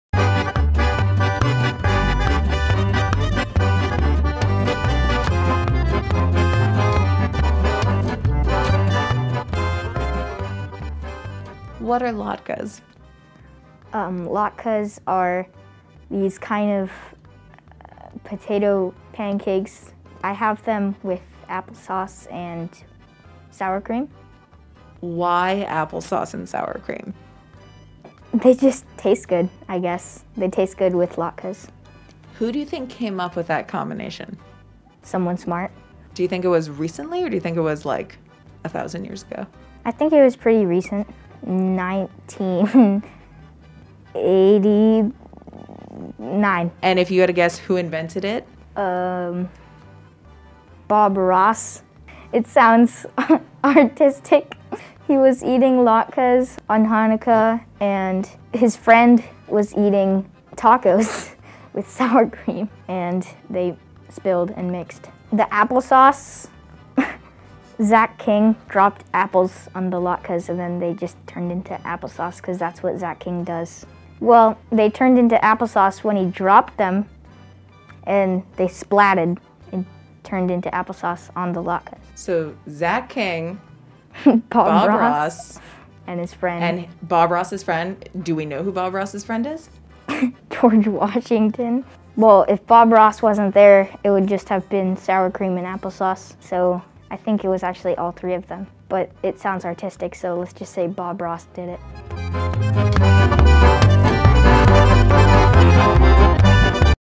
We Asked Our JCC East Bay Afterschool Students About Chanukah...
EDITOR:
Dane Fox-McGraw

INTERVIEWER:
Brianna Sommer

PRODUCER, DIRECTOR & EDITOR:
Eli Raskin